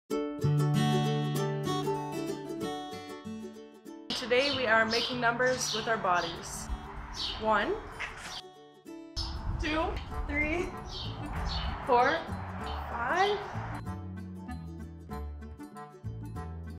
NLPS Summer Bingo - Numeracy - Making Numbers with Your Body
Join our student volunteers as they complete the activities in our Summer Bingo Challenge. Today's challenge: Use your body to form the numbers 1 to 5.